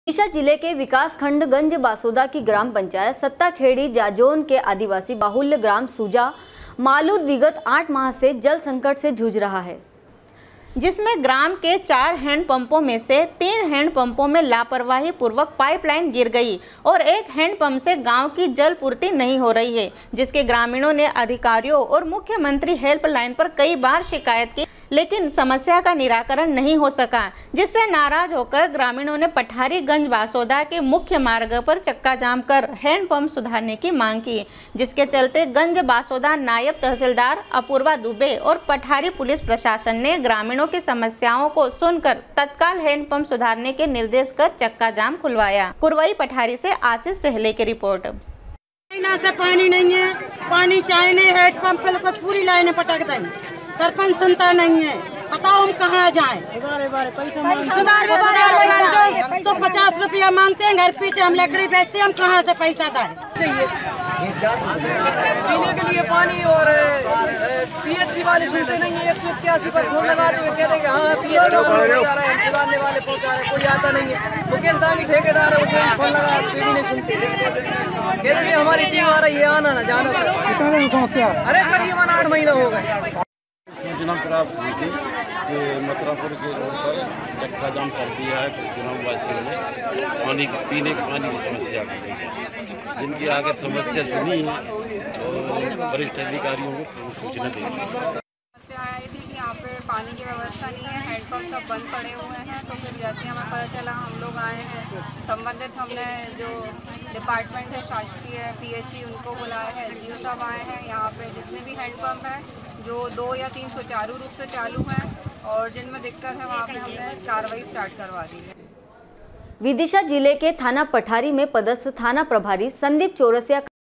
SW 24 News Madhyapradesh vidisha 22.07.19
जिला विदिशा के विकासखंड गंजबासौदा की ग्राम पंचायत सत्ताखेड़ी जाजोन के आदिवासी बाहुल्य ग्राम सूजा मालूद विगत 8 माह से जल संकट से जूझ रहा है।